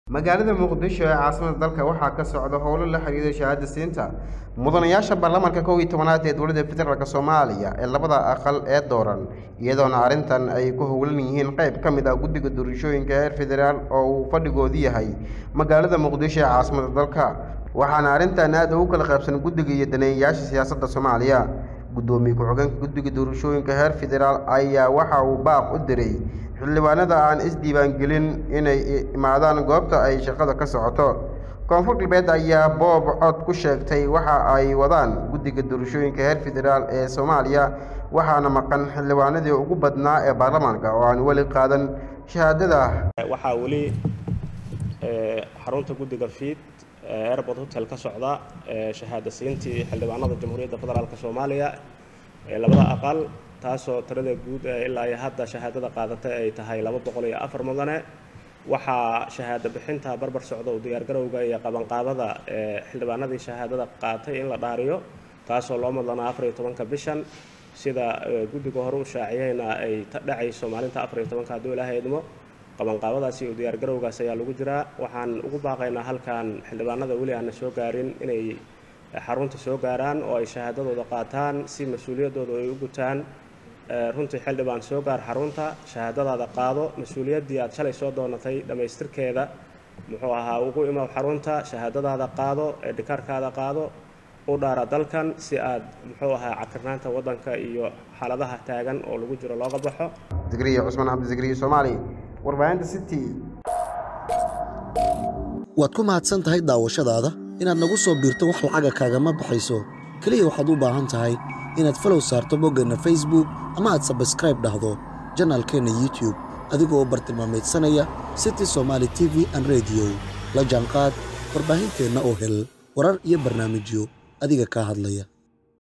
"Waxaan ugu baaqeynaa xildhibaanada aan Weli soo gaarin ee Maqan inay Xarunta Soo..."
"Waxaan ugu baaqeynaa xildhibaanada aan Weli soo gaarin ee Maqan inay Xarunta Soo gaaraan oo ay Shahaadadooda qaataan" Gudoomiye Kuxigeenka Guddiga Doorashada Heer Federal.